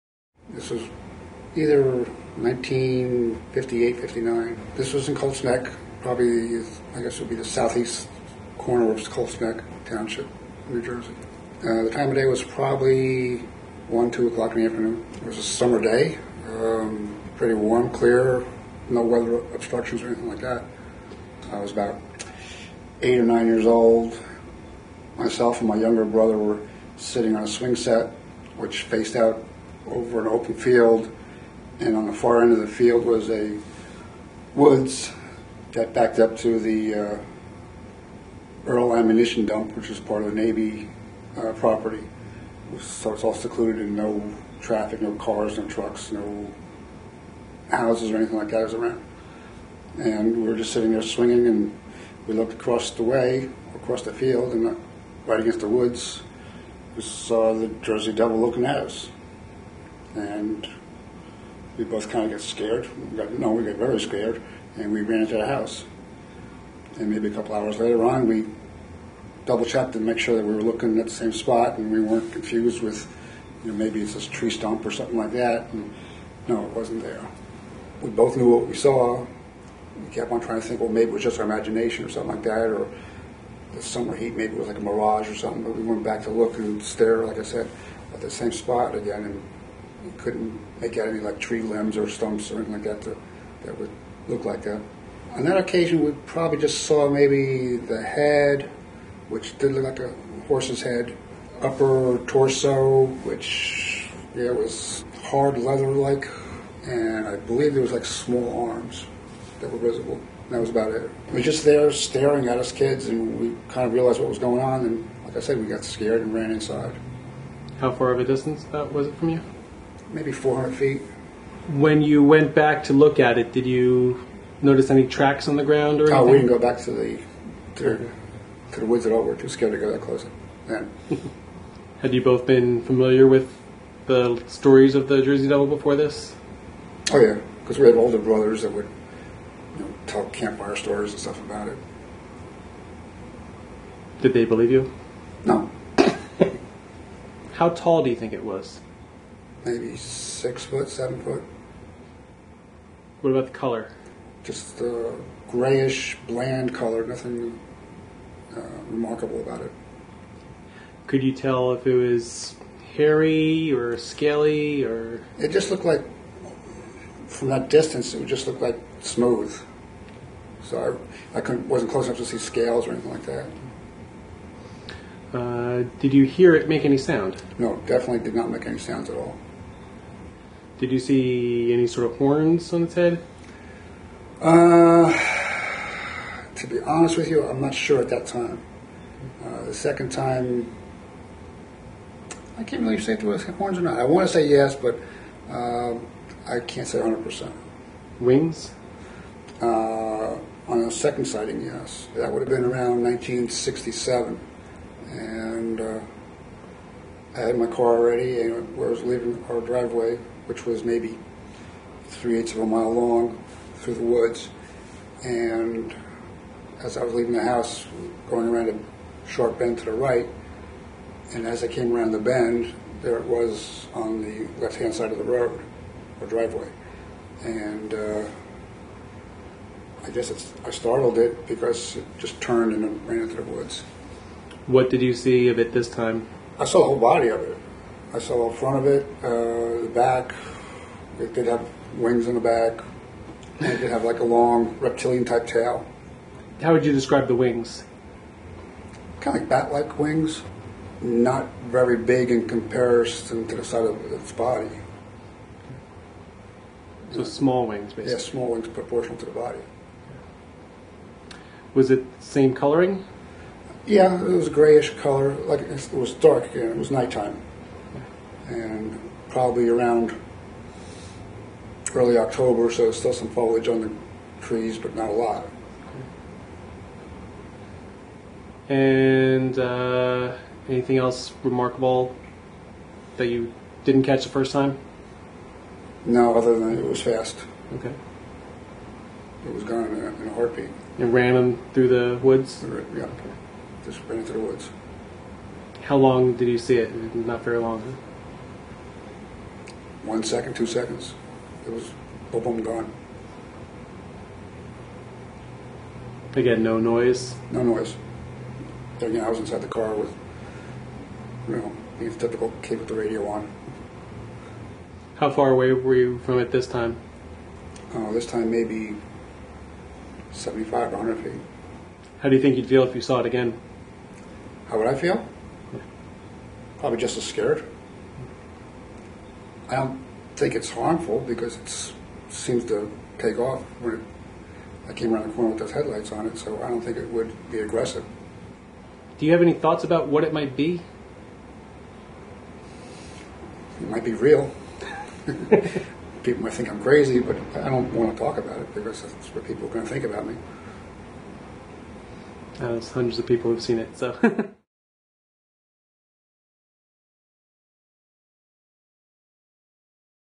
The Devil Hunters Eyewitness Interview: Marty Piatkowski
The Devil Hunters present an interview with an eyewitness who has encountered the Jersey Devil on two separate occasions - once in 1958-59, then again in 1967, both at his home in Colts Neck, New Jersey.